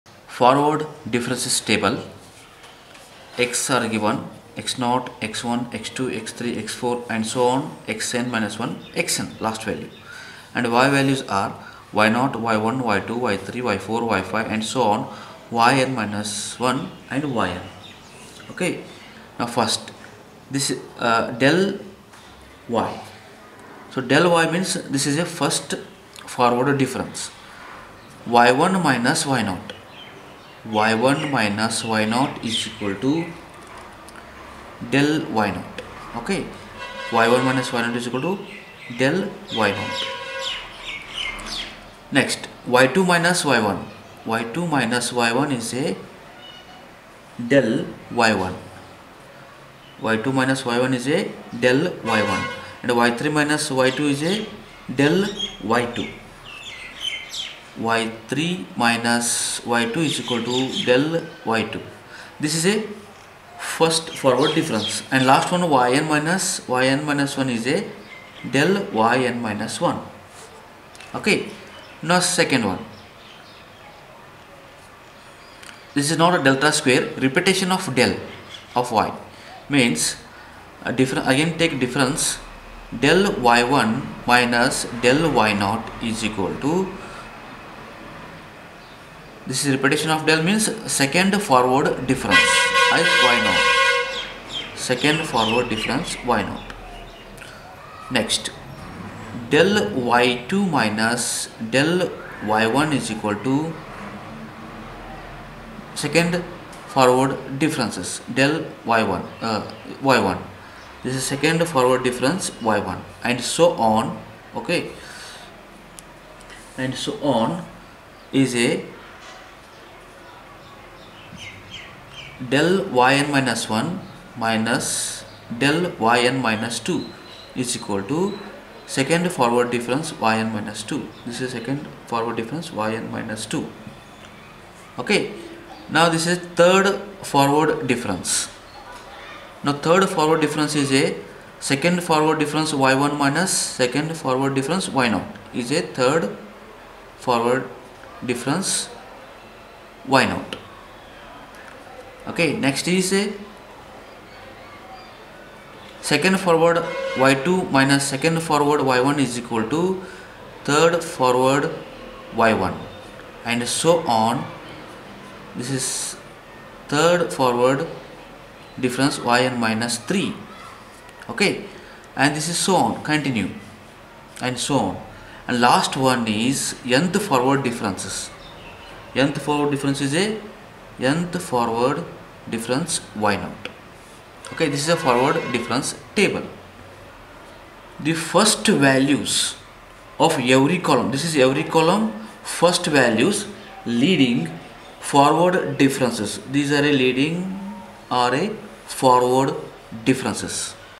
Forward differences table very simple concept
In this video explaining one interesting method forward difference table. This method is very simple. This method helps engineering students and basic science students.
The forward difference method is commonly used in numerical analysis and interpolation to approximate derivatives of functions and to construct polynomial approximations to functions. It is also used in finite difference methods for solving differential equations.